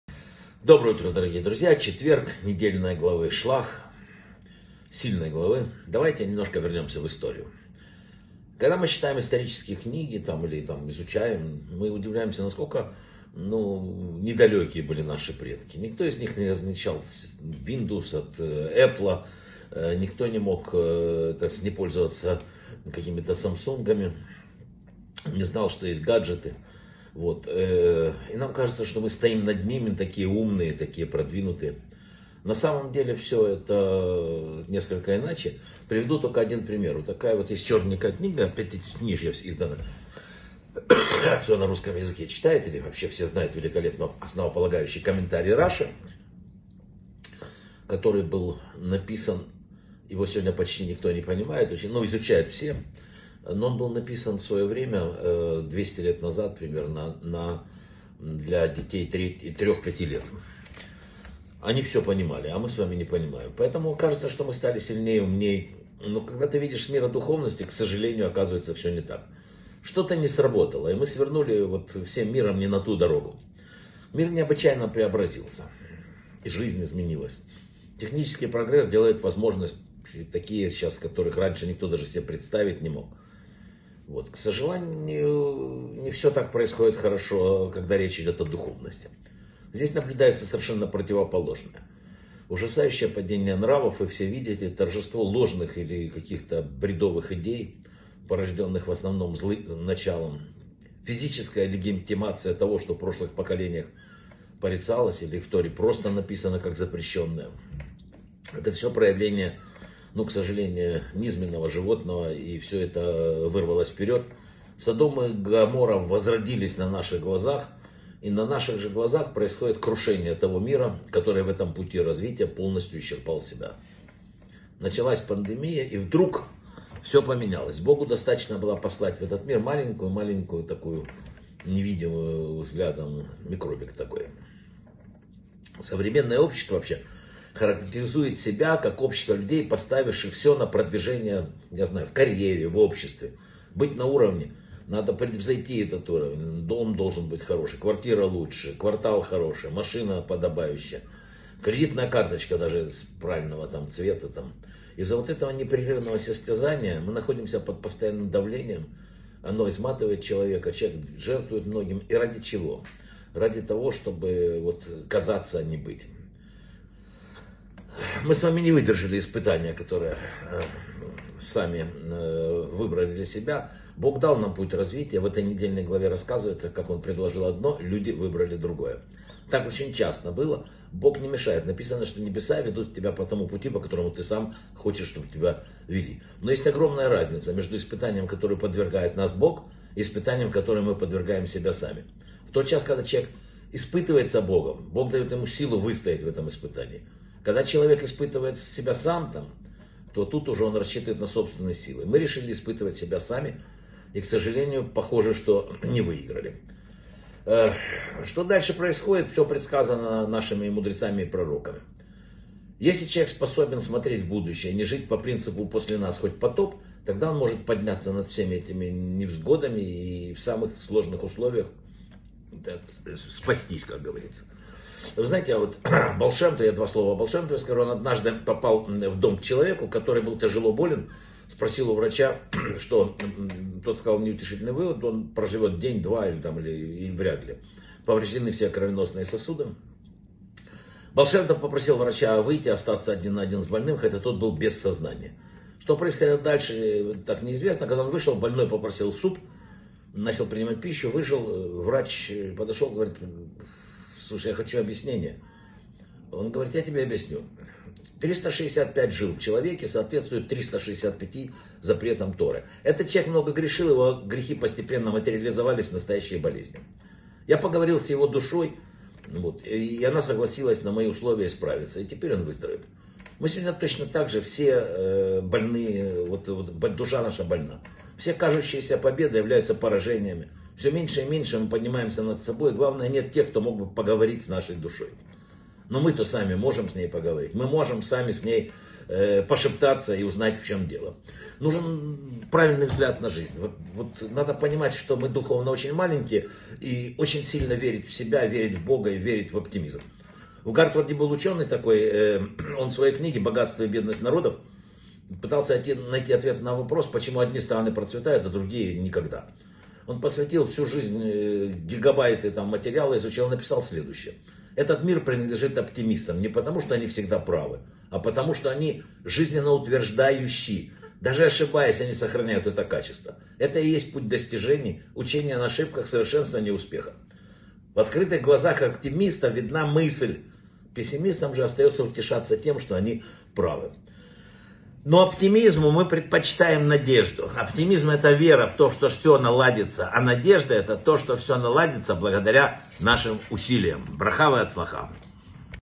📘 История повторяется - ведь люди не меняются... Недельная глава Шлах | Вадим Рабинович
История повторяется - ведь люди не меняются...

Почему сегодняшнее поколение на самом деле не сильнее и не умнее прошлых?
Какие проявления злого начала указывают на то, что мы идем не тем путем?
Какую ошибку, на которуюнамекает наша недельная глава, человечество совершает снова и снова?
В чем разница, между испытаниями, посланными Б-гом и теми, которым мы подвергаем себя сами?
Какой человек способен подняться даже в самых сложных условиях?
История о Баал Шем Тове и исцелении больного.
В чем отличие оптимизма от надежды и почему миром владеют оптимисты?